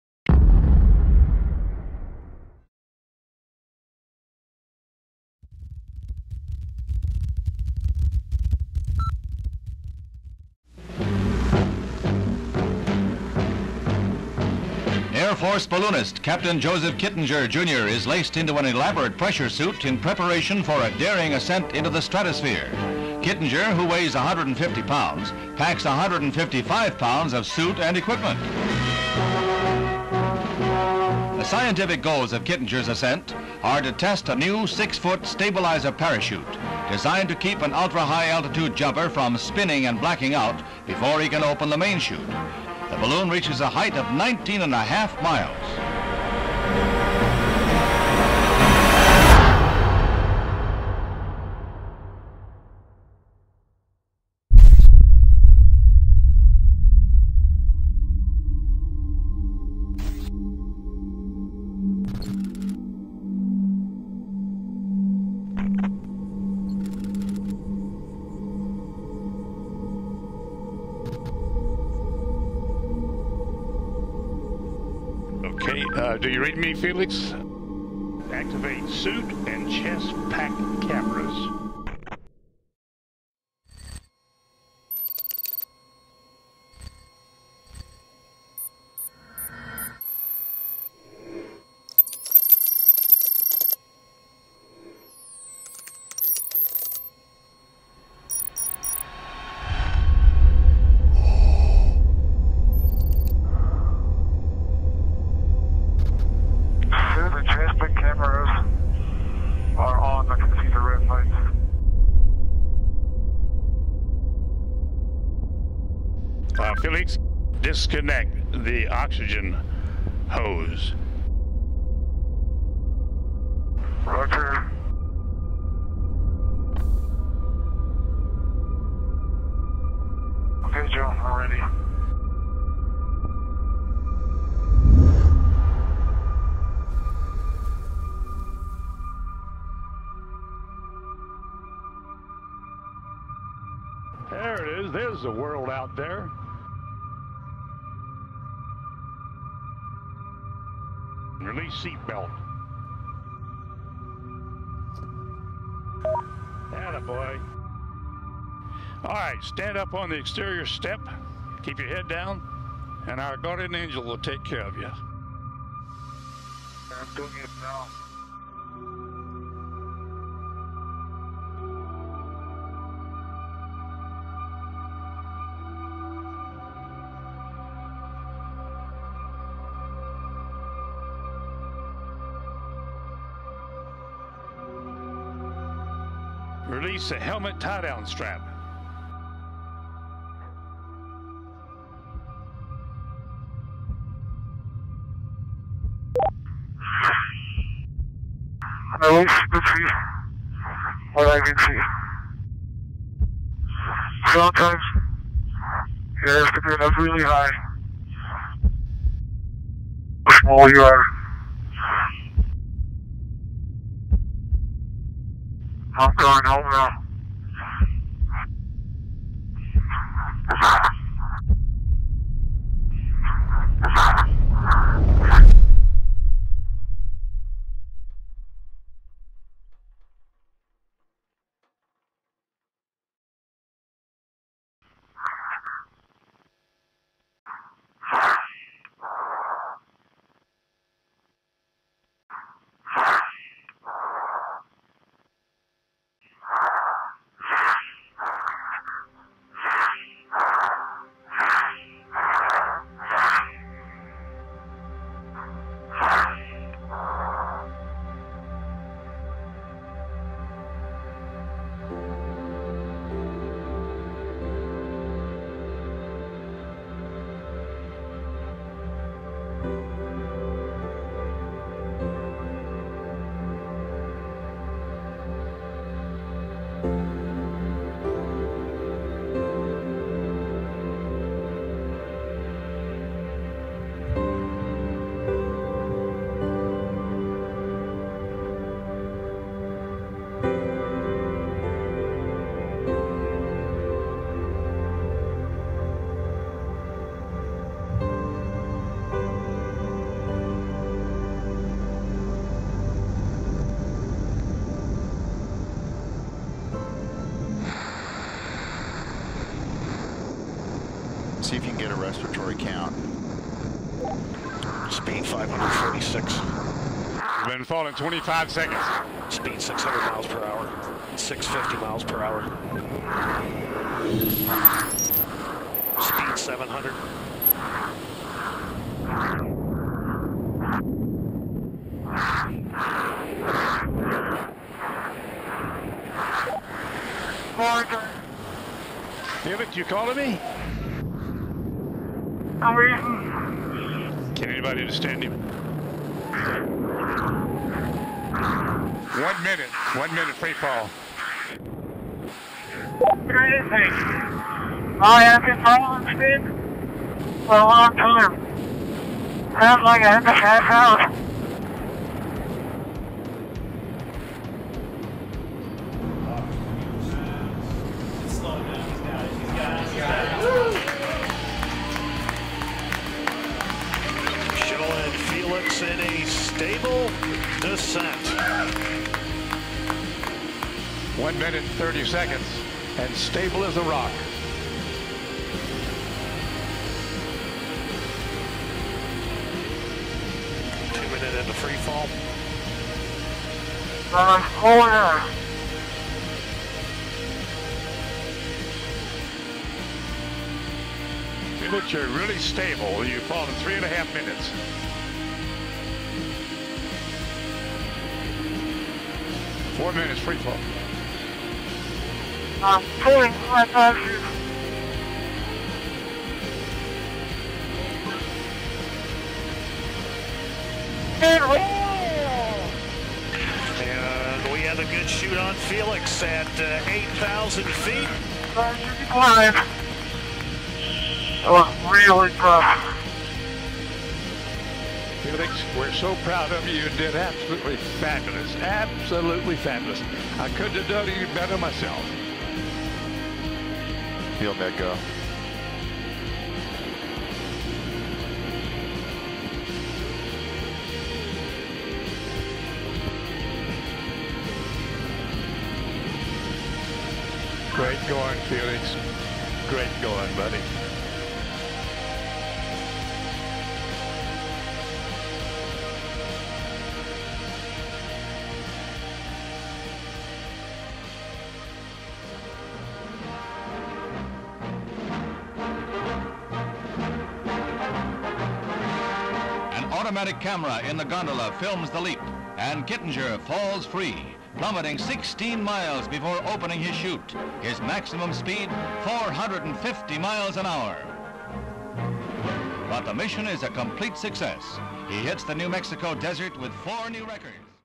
Felix Baumgartner'in uzay atlayışı
Felix Baumgartner’ın uzay atlayışında 4 dakika 20 saniye içerisinde 1000 km/saat  hıza ulaştığı atlayışının kamera görüntülerini mutlaka izlemelisiniz.